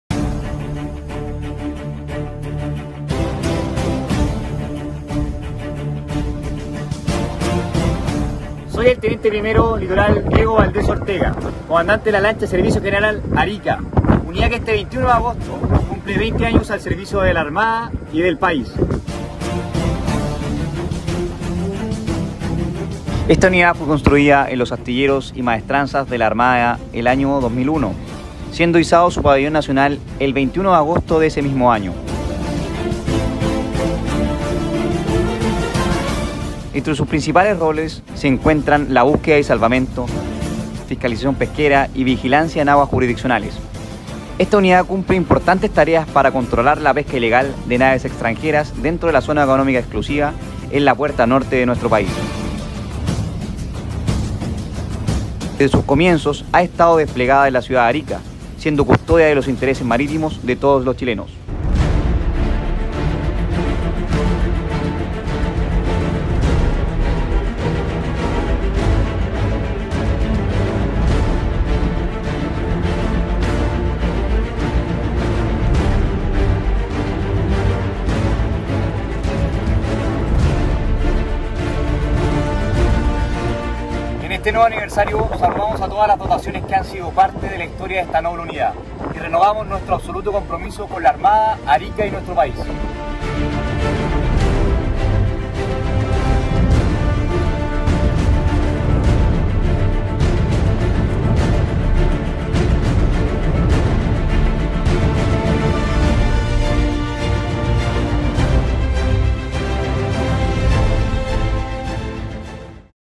Aniversario LSG-1615 "Arica"
ANIVERSARIO LSG “ARICA”

Hoy saludamos a todas las dotaciones que han sido parte de esta noble unidad en sus 20 años al resguardo de la puerta norte de nuestro país.